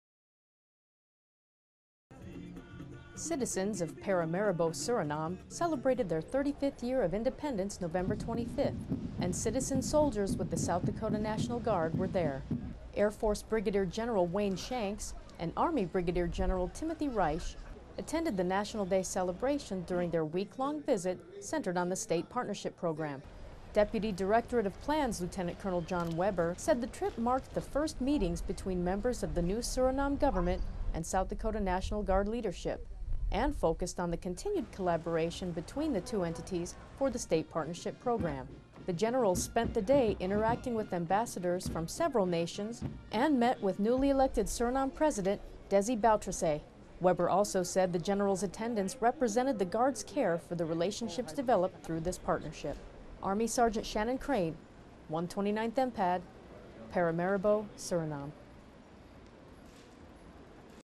SDNG Senior Leadership Participate in National Day
By:  SSG Shannon Crane -- Senior leadership with the South Dakota National Guard participated in the National Day celebration during their visit to Paramaribo, Suriname, November 2010.